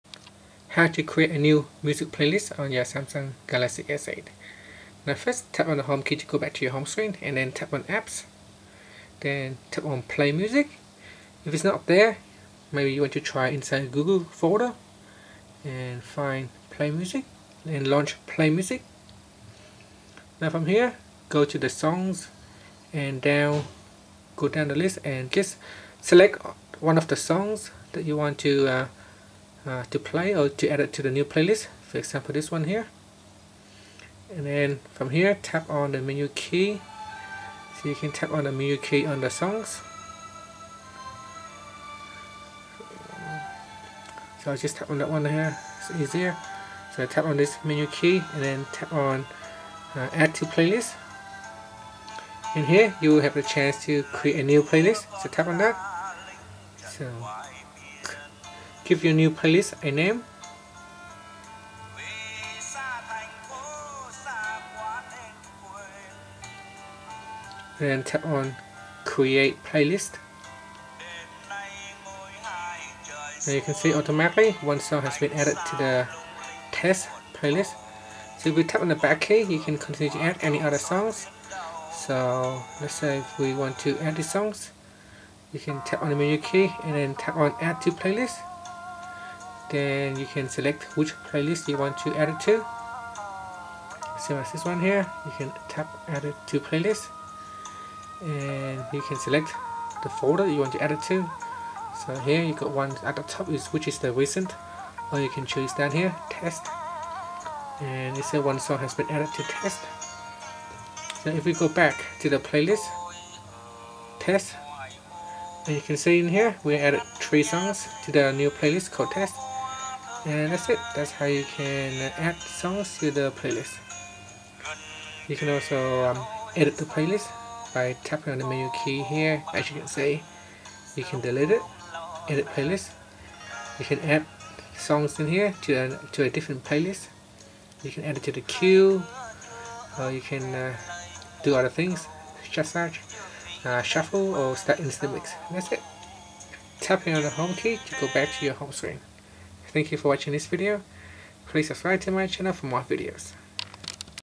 Samsung Galaxy S8: How to Create New Music Play list
Learn how you can create a new music playlist on Samsung Galaxy S8.